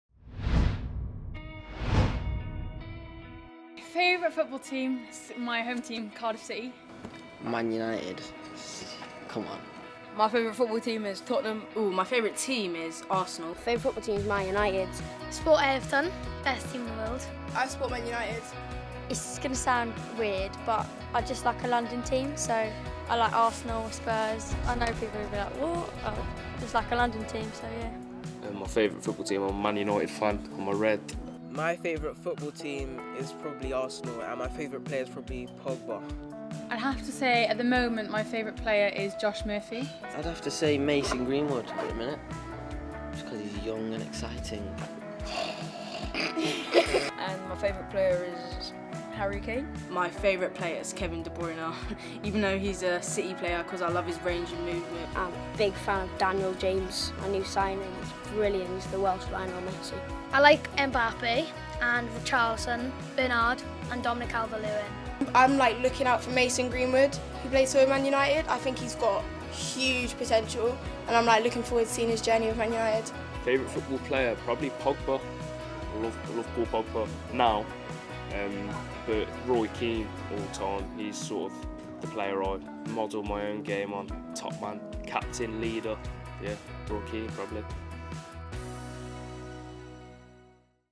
JAMIE JOHNSON - CAST QUESTION TIME: WHO'S YOUR FAVOURITE FOOTBALL TEAM??
The Jamie Johnson cast answer the two big questions we all want to know.. Who is your favourite football team and which player do you like at the moment?